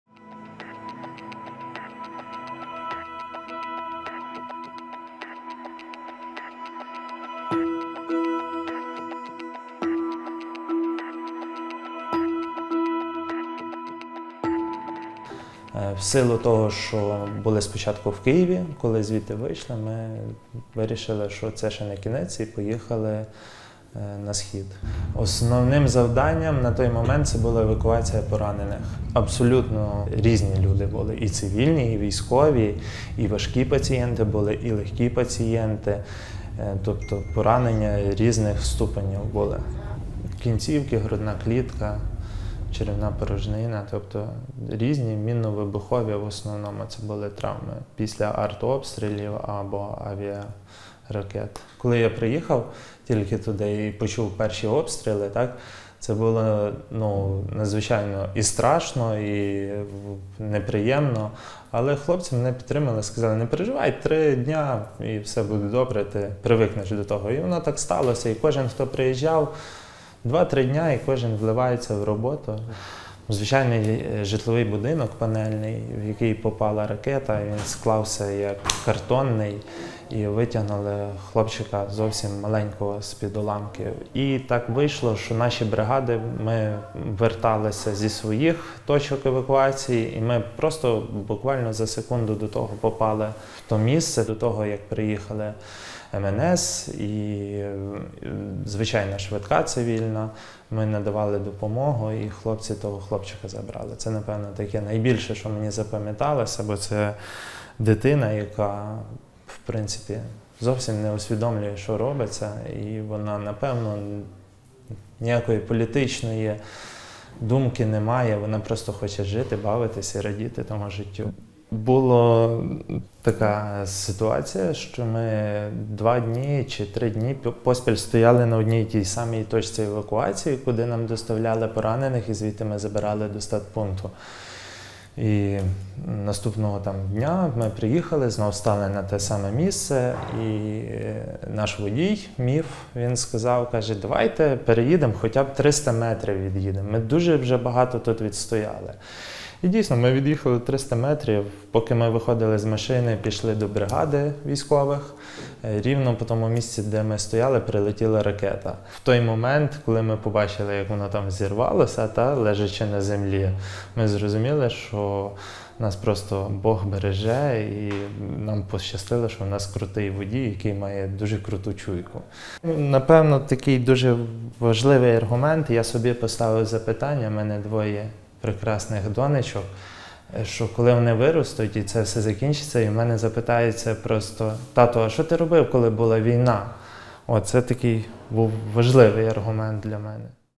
'We Took A Baby From The Rubble': A Ukrainian Medic's Tale
A Ukrainian trauma doctor says it took a few days to get used to the carnage after volunteering to serve on the front line. Danylo Sereda says his strongest memory was dealing with a baby in a house that "folded like a house of cards" after a Russian rocket attack.

Check (and bookmark) our regularly updated briefing on the latest situation and outstanding reporting